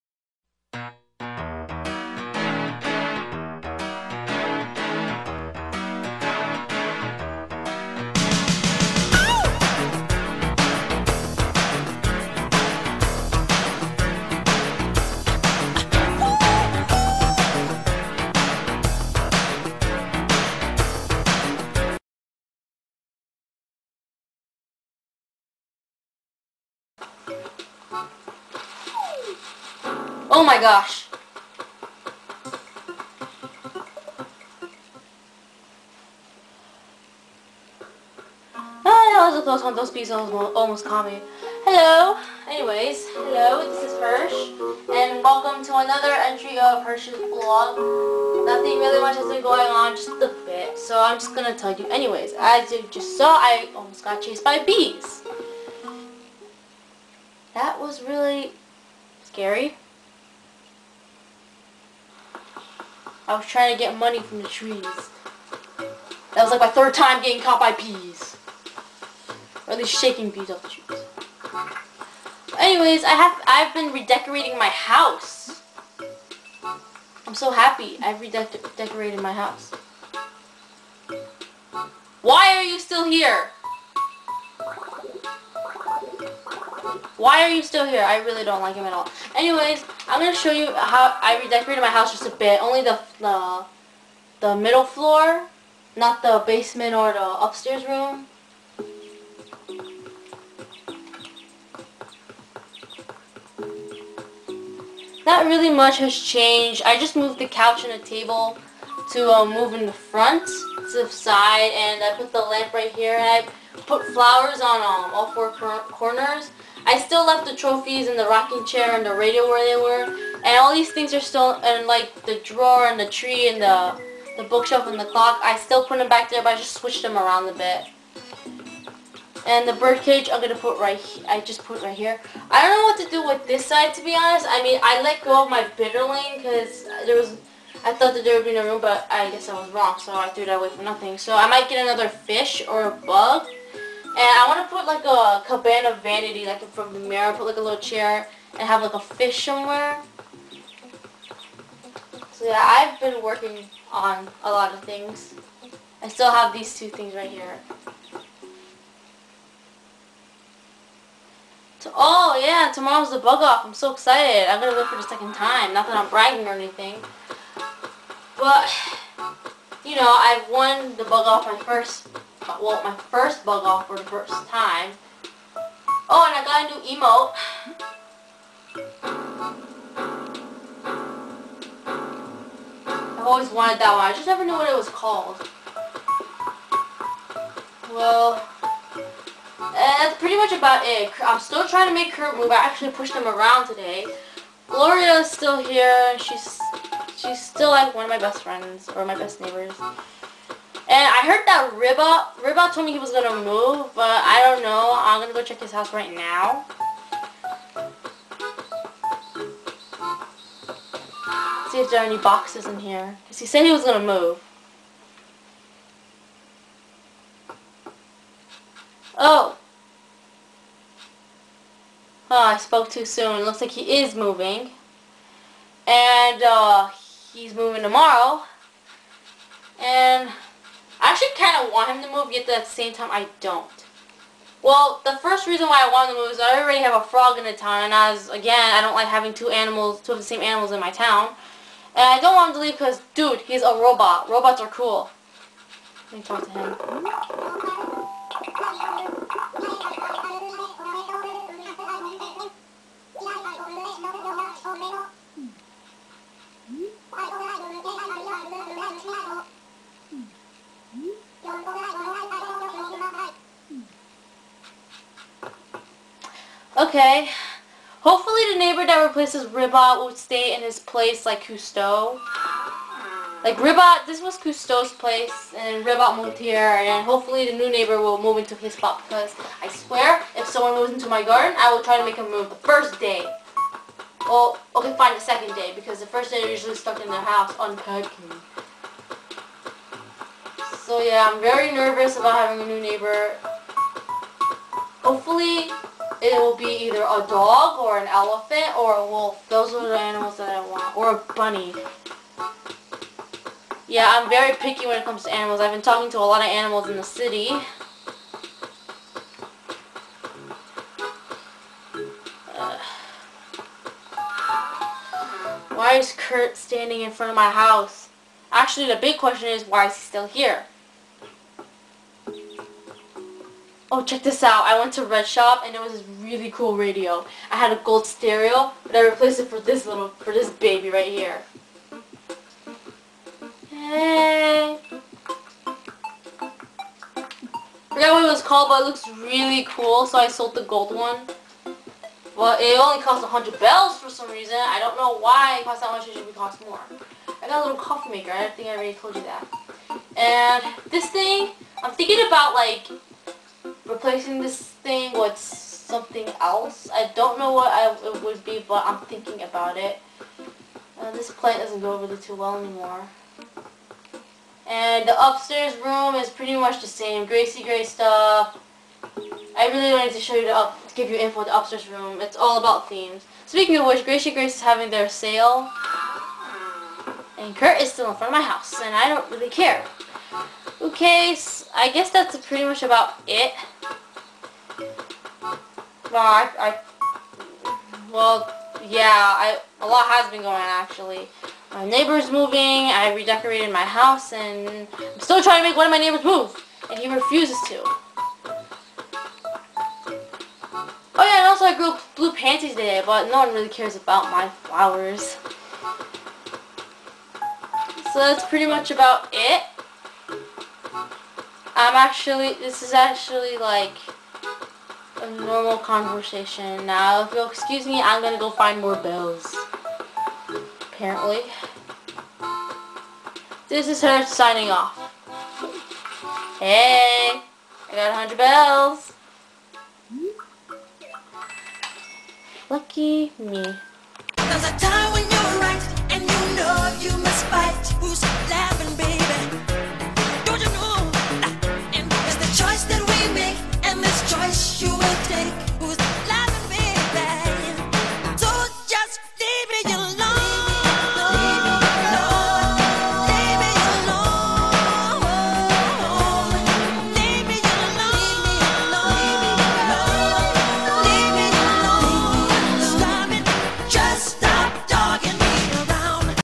Hersh's Blog 7/17/09 - A Little Change is Good
Today, Hersh is all about redecorating her house while discovering about one of her neighbors in time and finding bells off trees!

PLEASE DO NOT PUT ANY COMMENTS ASKING ME TO COME TO MY TOWN OR FOR MY FRIEND CODE!! I'M SICK AND TIRED OF HAVING TO REPEAT MYSELF AND IT'S ANNOYING! I WILL SAY NO!